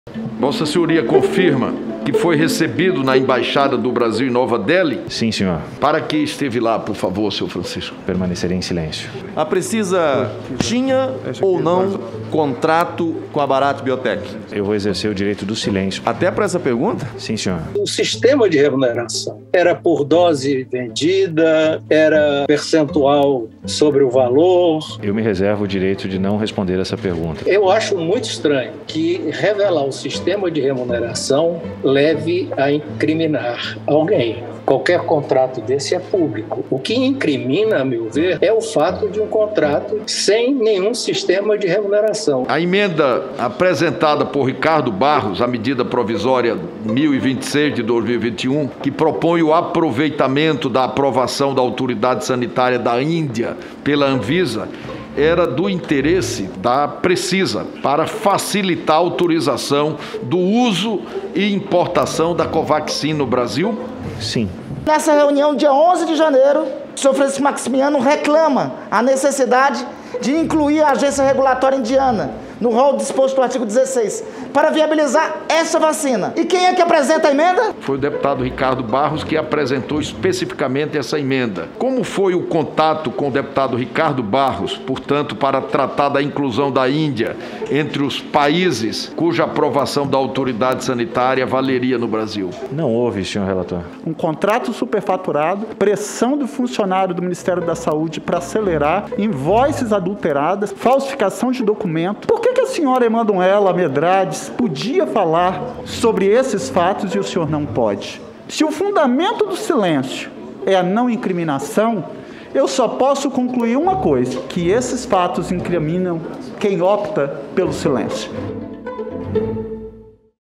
Dono da Precisa Medicamentos silencia em depoimento e irrita senadores na
Blindado por um habeas corpus, o dono da Precisa Medicamentos, Francisco Maximiano silenciou diante das perguntas e irritou senadores na CPI. "Se o fundamento do silêncio é a não incriminação, eu só posso concluir uma coisa: que esses fatos incriminam quem opta pelo silêncio", questionou o senador Fábio Contarato.